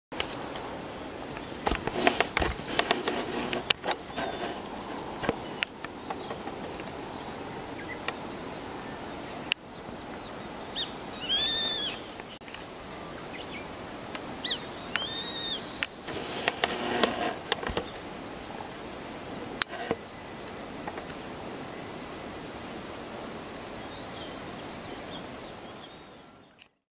220709 サシバ
2022/07/09 新潟県魚沼市山崎遊歩道にて収録
Place: Uonuma City, Niigata Prefecture, Japan

かなりブレています。失礼。
Sorry for the shake of the image.

二番目の動画の背景は越後駒ヶ岳。
Mt. Komagatake of Echigo is on the background in the second segment.